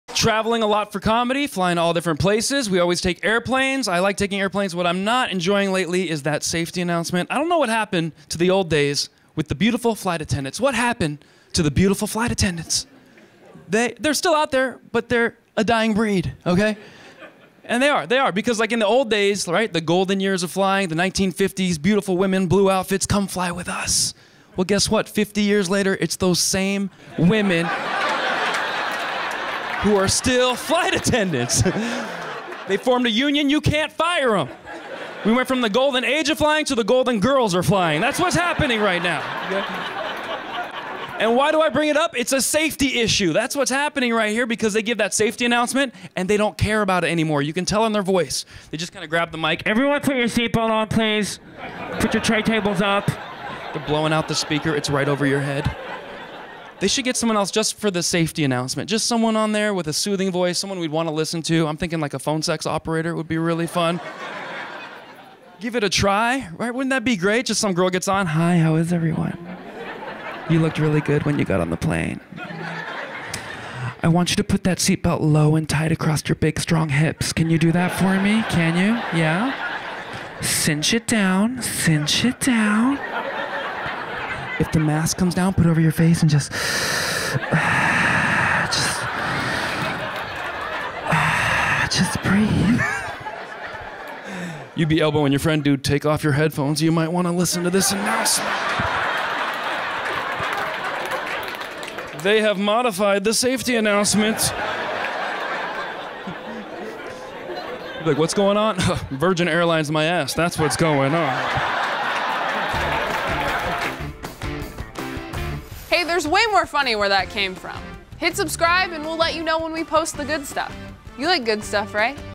Old Flight Attendants | K-Von | Stand-Up Comedy
Comedian K-Von talks about how traveling can get old quick.